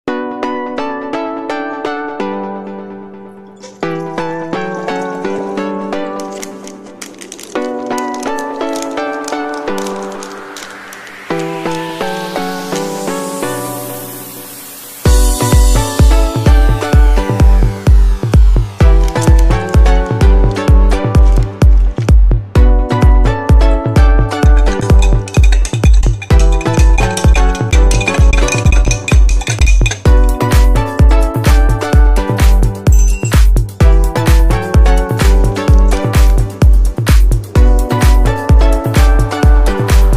Garcinia Cambogia || How to Prepare
Guaranteed Effective Slimming Grape Juice

1. Water first
2. Empty sachet
3. Mix well
4. Put ice if desired
5. Enjoy your slimming grape juice drink

Drink every morning, in empty stomach 🍷